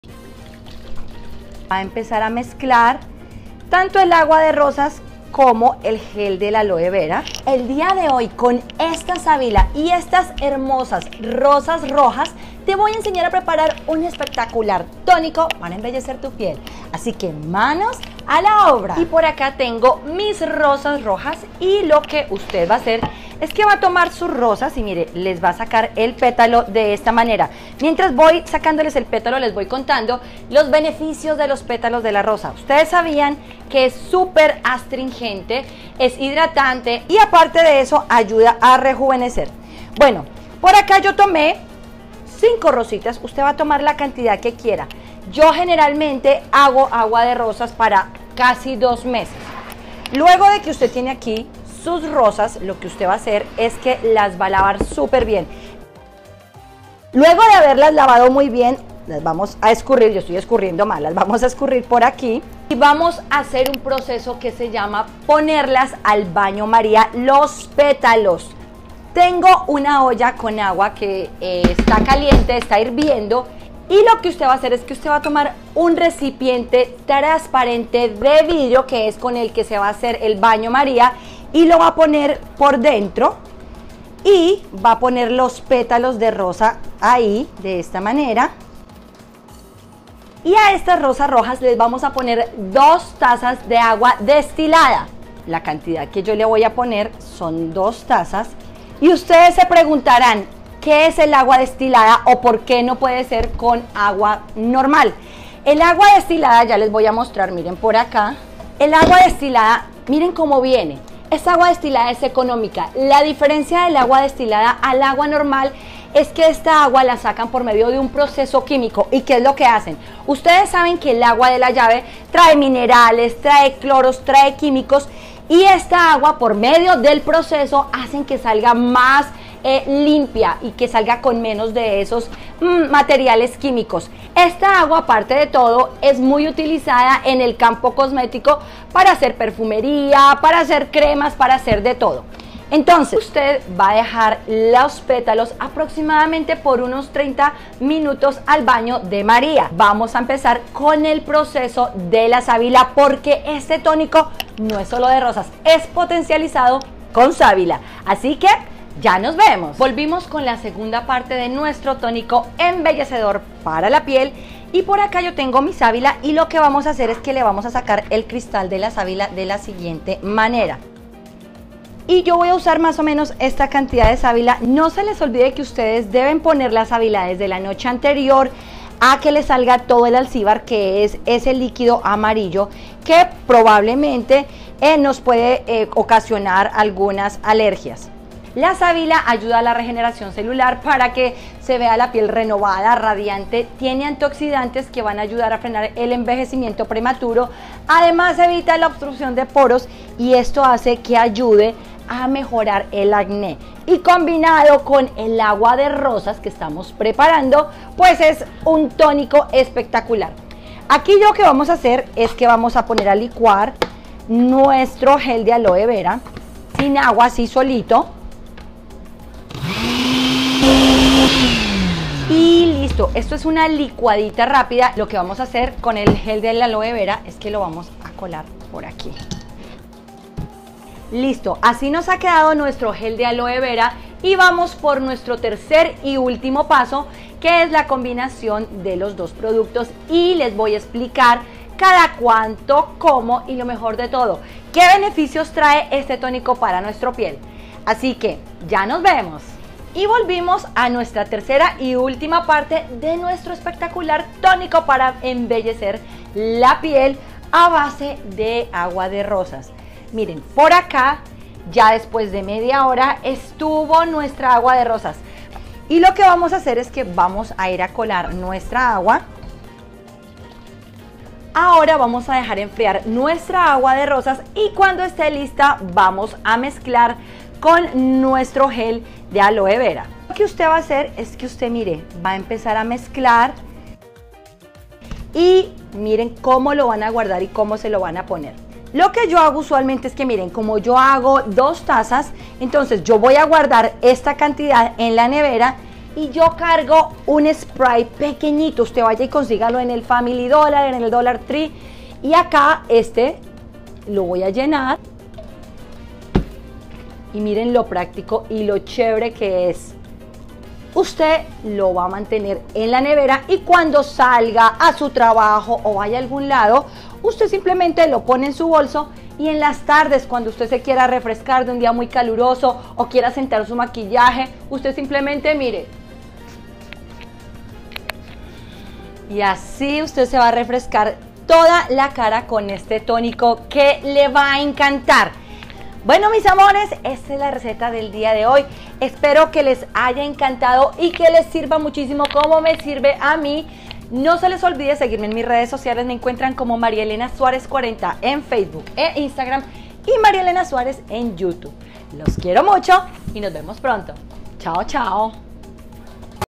COMO LUCIR UNA PIEL RADIANTE CON ESTE TONICO DE ROSAS
COMO LUCIR UNA PIEL RADIANTE CON ESTE TONICO DE ROSAS. Este espectacular tónico te hará lucir un rostro más saludable, brillante y sano. Anímate a hacerlo te sorprenderá.

¡Conéctate conmigo!